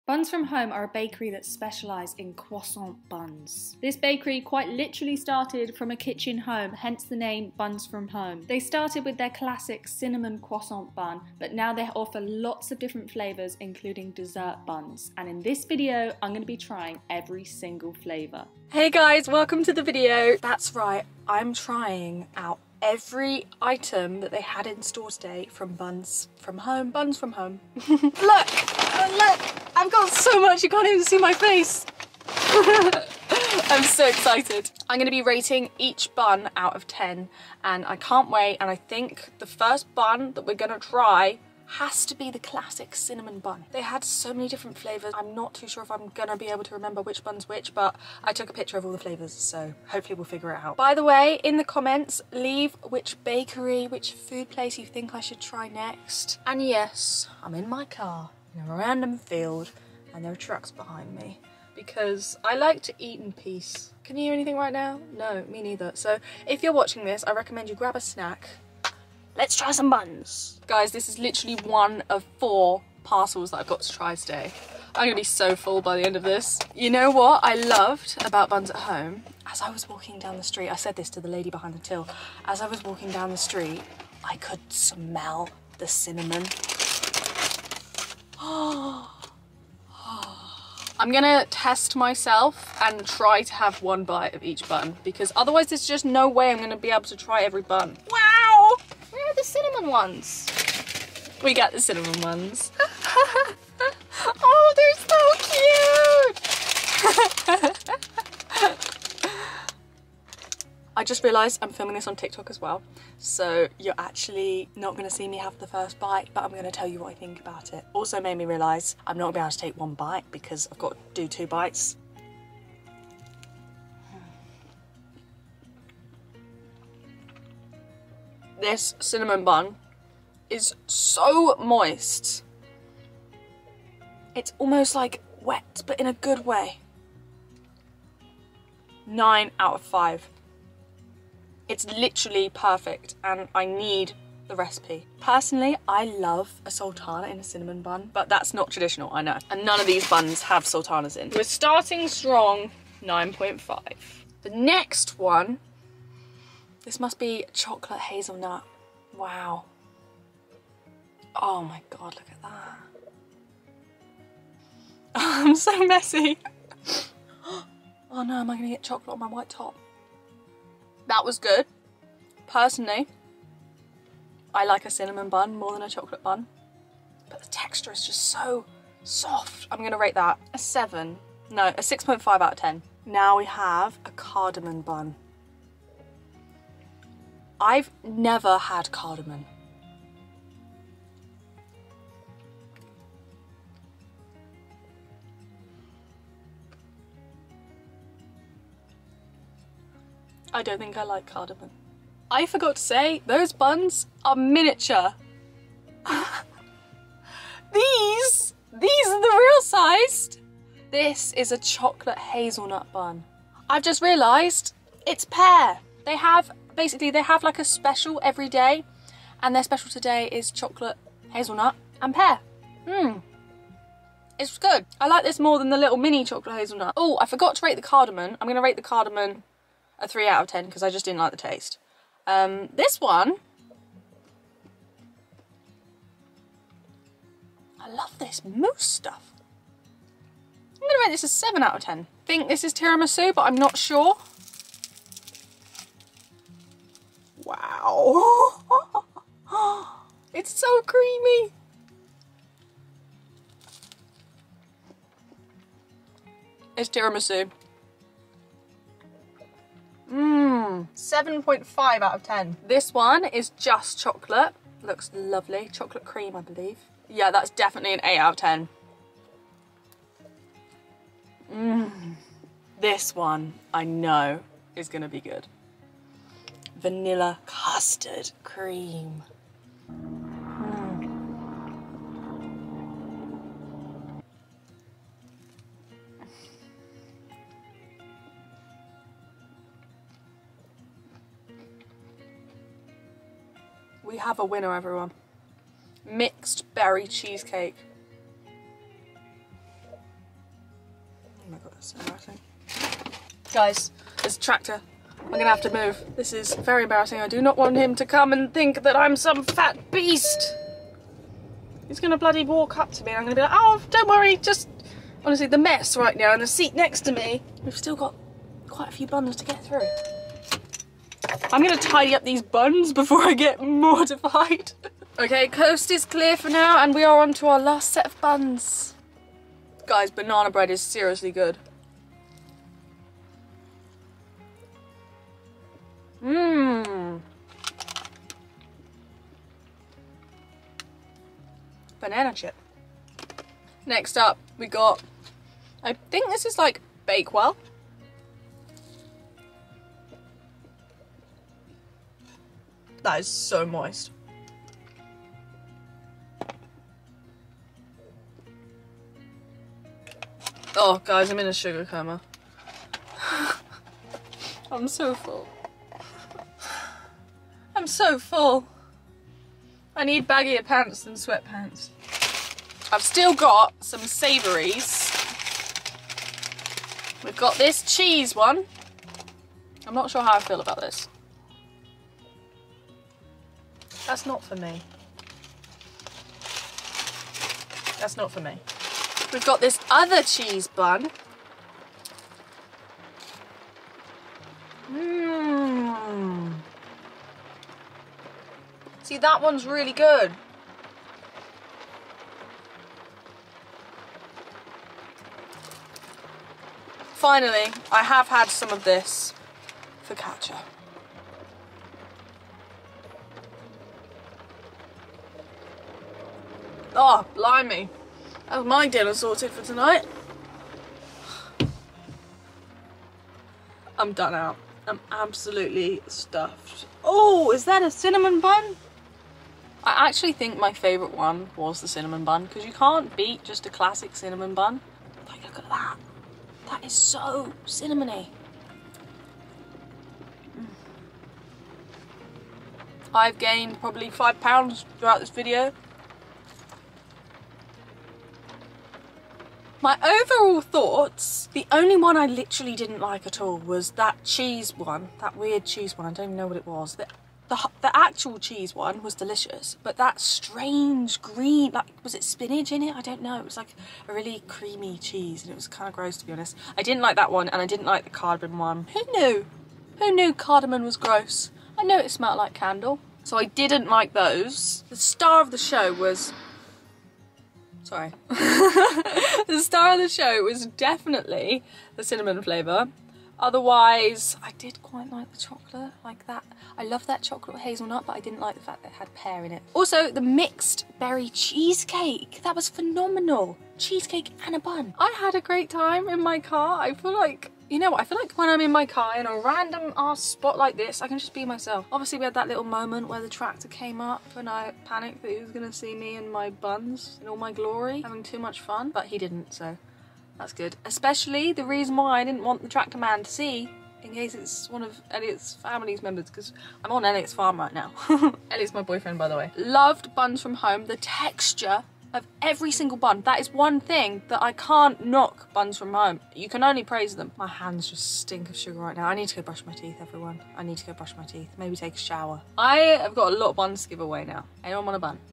Trying everything from BUNS FROM HOME | London Bakery taste test
I had the BEST time in London, and I would highly recommend buns from home for a sweet treat! Hope you enjoyed the video (I certainly enjoyed making it haha, editing it made me hungry though...)